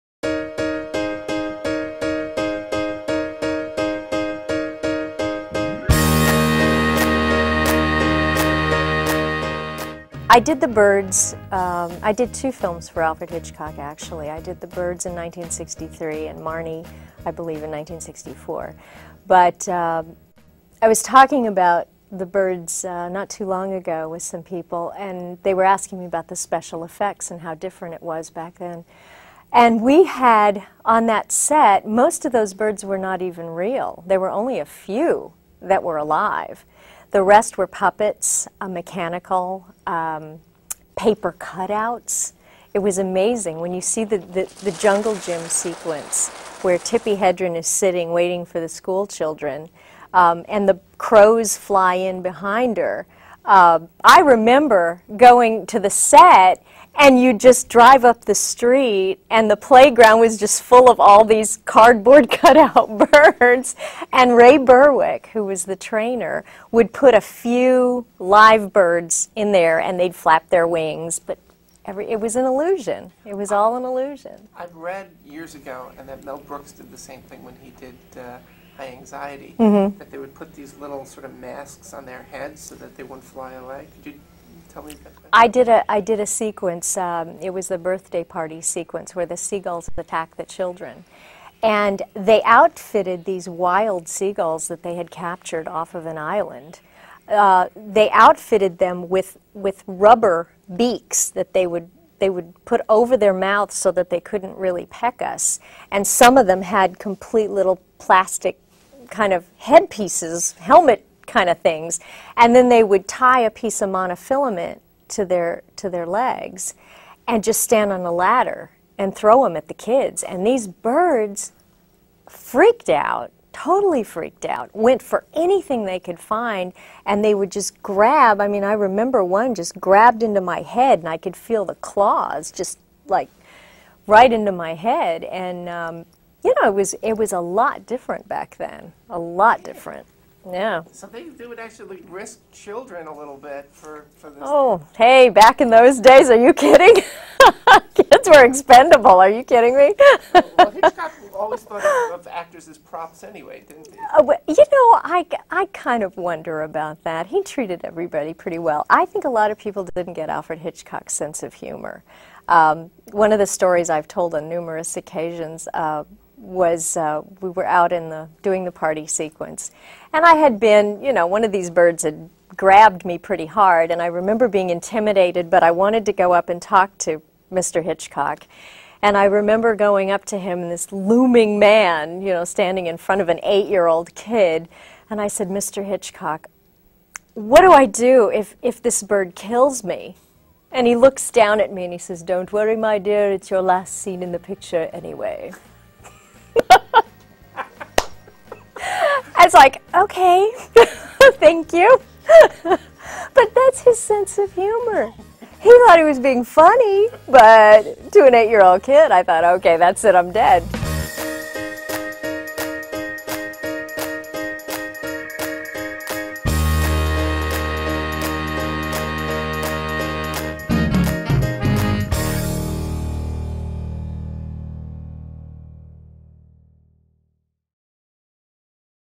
The Secrets of Alfred Hitchcock's The Birds Revealed by Morgan Brittany
Morgan Brittany remembers how, as a child, she appeared in Alfred Hitchcock's THE BIRDS.  She reveals how some of the chilling special effects were achieved.